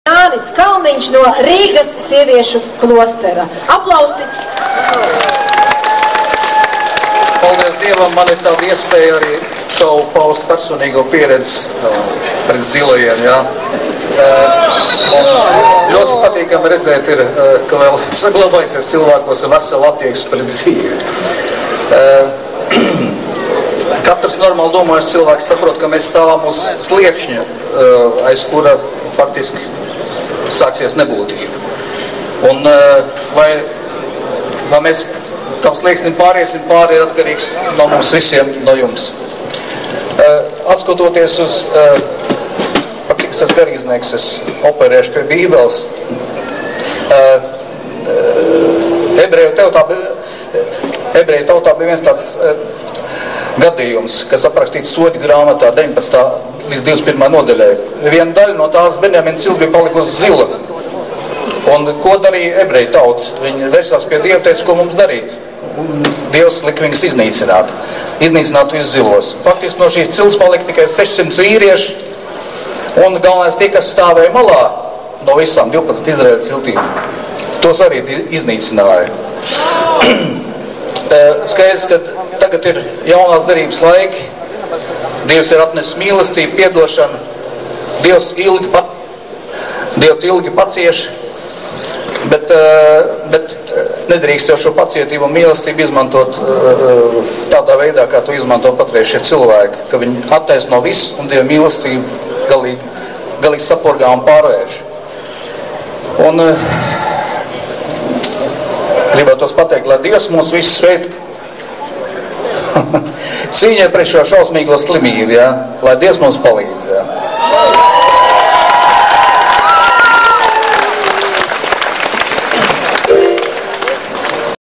2007-03-06 J. Kalniņa uzruna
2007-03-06 J. Kalniņa uzruna pasākumā "Pasaule pret homoseksuālismu".